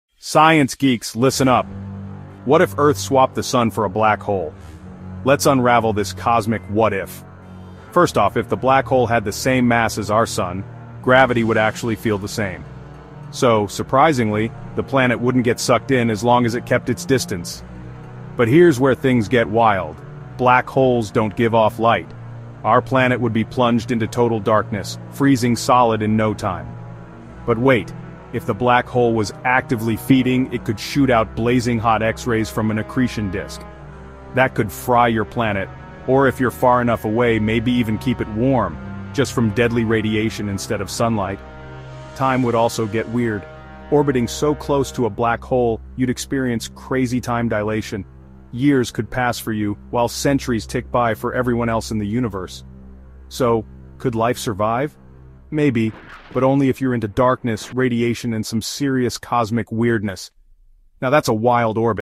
What If a Planet Orbited a Black Hole?
Imagine a world circling the edge of darkness…
If a planet orbited close to a black hole, time would slow down, light would bend, and reality itself would warp. Could life survive? Could we live there? In this short, we explore the terrifying beauty of a planet dancing with gravity’s most powerful force.